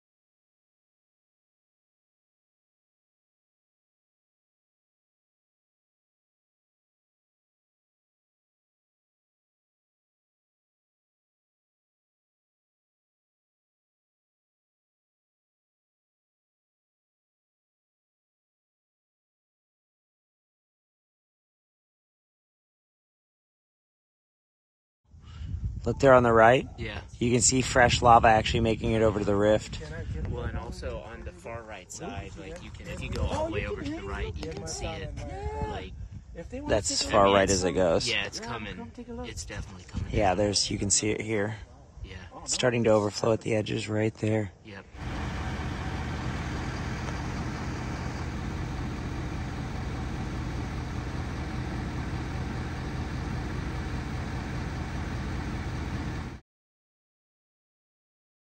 World’s largest active volcano Mauna Loa erupts in Hawaii
The world's largest active volcano, Mauna Loa, has erupted for the first time in almost 40 years. The volcano's alert level has been upgraded from "advisory" to "warning", the highest classification.